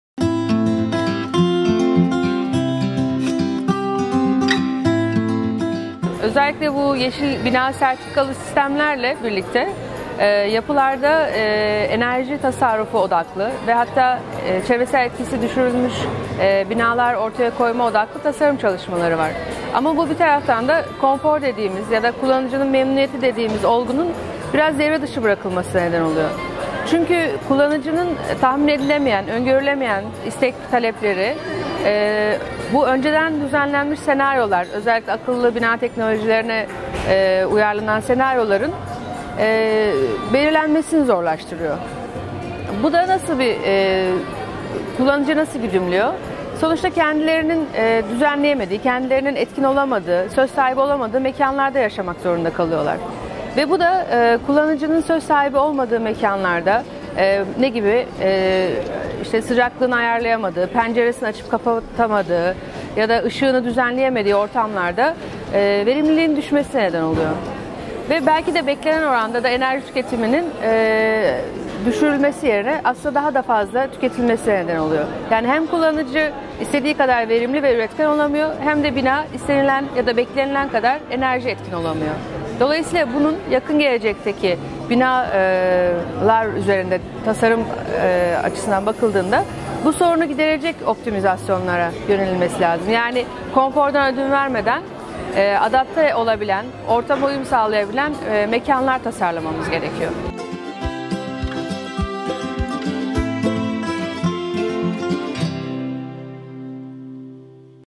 Geleceğin Binaları Nasıl Tasarlanmalı? | Sürdürülebilirlik Adımları
Enerji tasarrufu odaklı, yeşil bina sertifikalı sistemlerin, akıllı binaların eksik kaldığı noktalar hakkında Özyeğin Üniversitesi Mimarlık ve Tasarım Öğretim Üyesi Özgür Göçer ile Yakın Gelecekte Binalar Konferansı'nda konuştuk. Devamı için